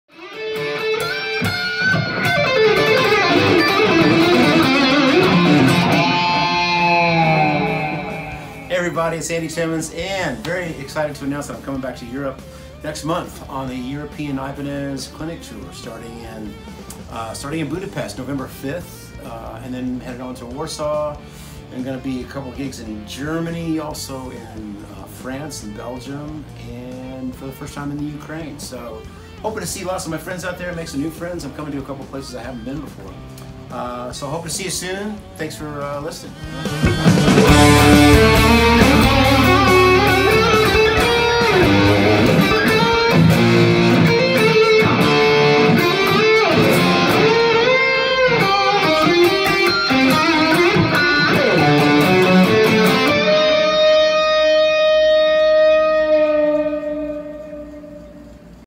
Andy Timmons - IBANEZ 2018 CLINIC TOUR UKRAINE.
Майстер-клас Енді Тіммонса у Києві
18 листопада о 18:00 у концертному залі Будинку Архітектора за адресою вул. Бориса Грінченка, 7

Вхід вільний за реєстрацією, кількість місць обмежена.

Енді Тіммонс (Andy Timmons) - легендарний соло артист і сесійний музикант, визнаний у всьому світі ще з кінця 1980-х нарівні з такими знаковими персонами як Steve Vai, Joe Satriani, Eric Johnson, Steve Morse, Mike Stern, Ace Frehley, Ted Nugent, і Pierre Bensusan. Крім маси музичних заслуг і нагород за внесок в музику, Енді є представником гітар Ibanez і підсилювачів Mesa Boogie.